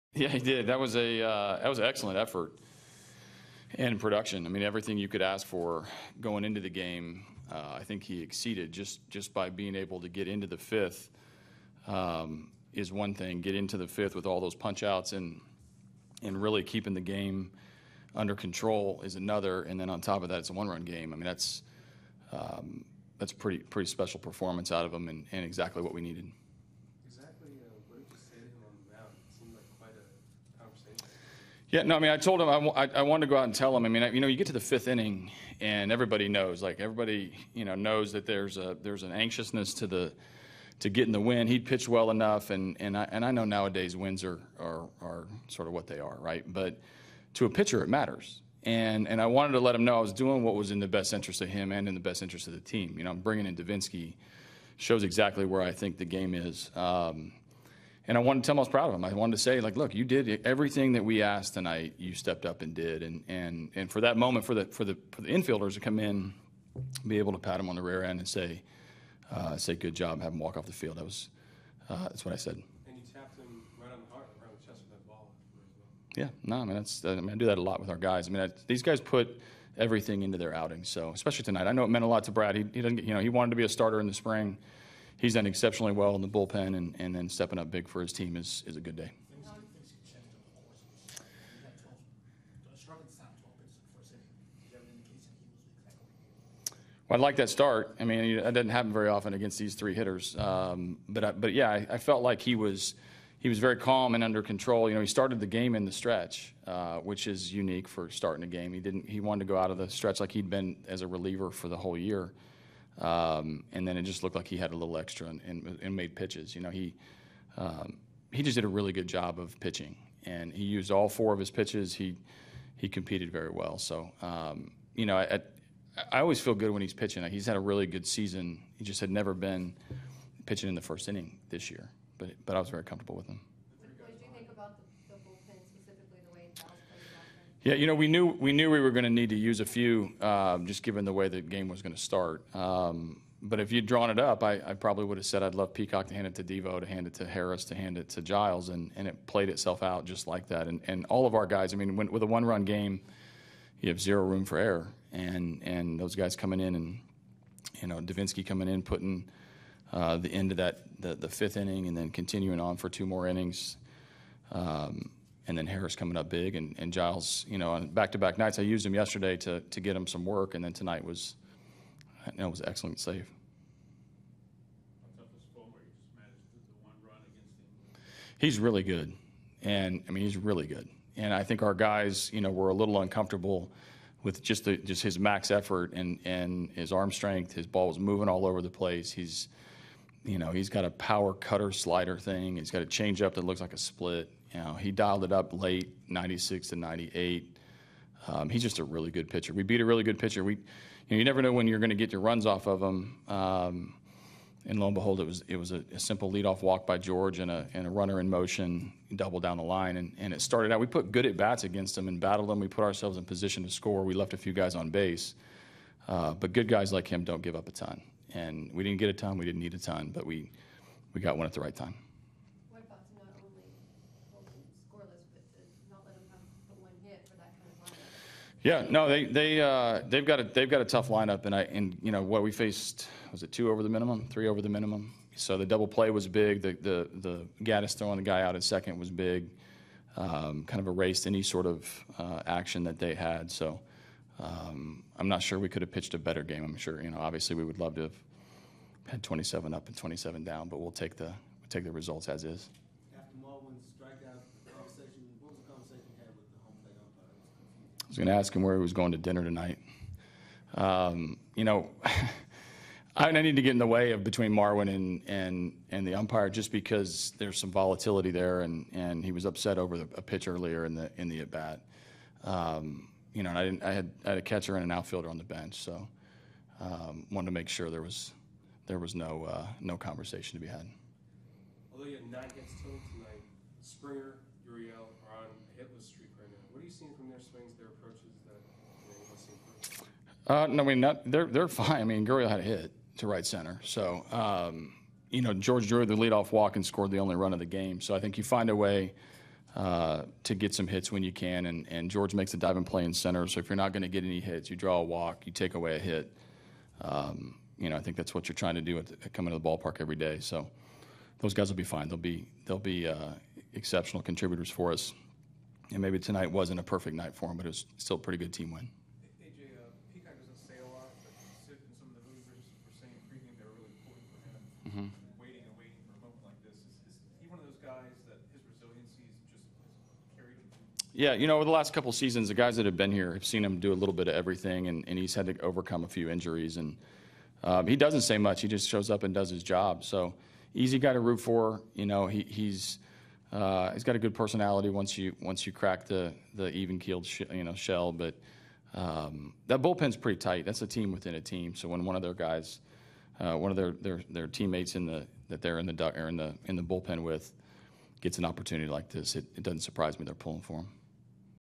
DET@HOU: Hinch on Peacock's impressive start
Astros manger A.J Hinch discusses Brad Peacock's impressive start in Houston's 1-0 win over the Tigers